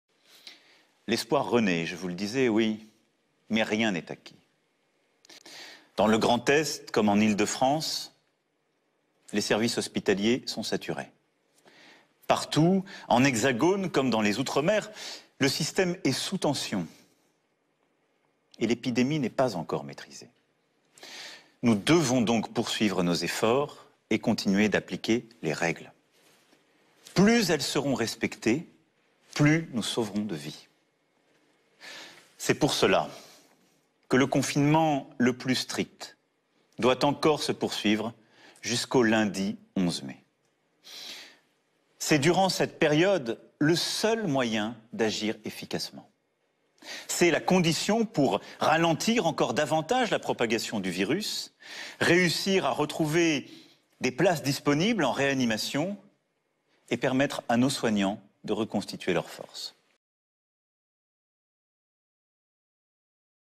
Coronavirus : Emmanuel Macron prolonge le confinement jusqu'au 11 mai
Emmanuel Macron s'est exprimé ce lundi soir au sujet de l'épidémie de Coronavirus qui frappe le pays. Dans son allocution le président de la République a annoncé la prolongation du confinement des Français jusqu'au 11 mai prochain. Les crèches, les écoles, les collèges et les lycées rouvriront progressivement. Les personnes âgées, en situation de handicap sévère ou atteintes de maladies chroniques, elles, devront rester confinées même après le 11 mai.